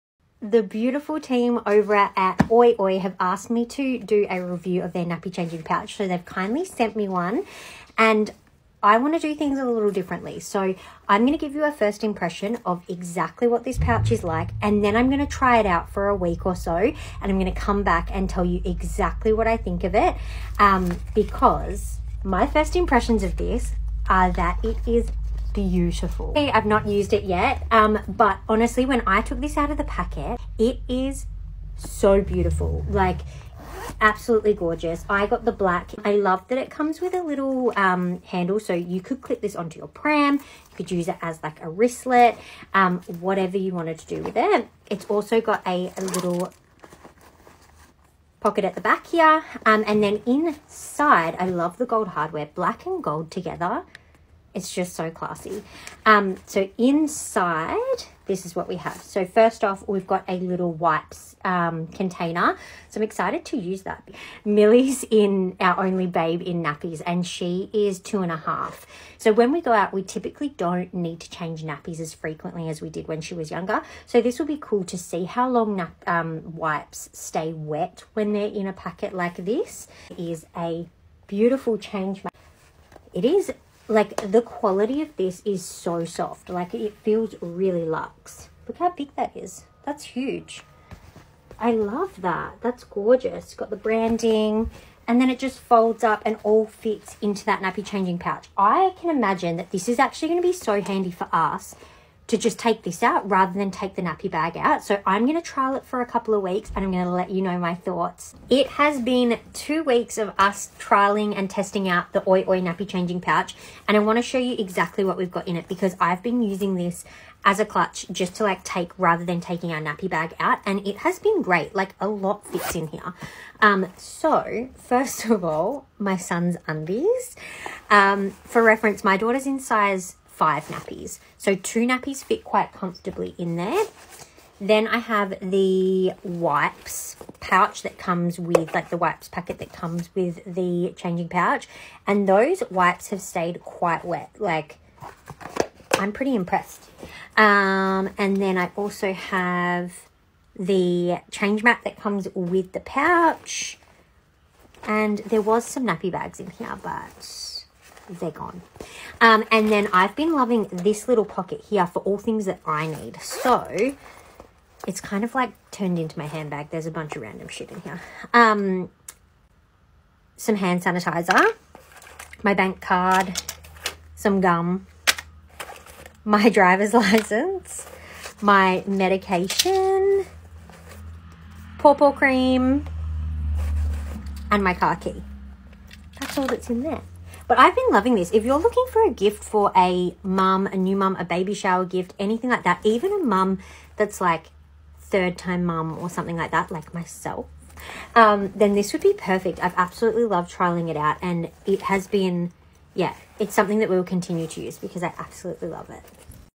I've been taking this out instead of my nappy bag!
OiOi Changing Pouch Review by @taylacespedes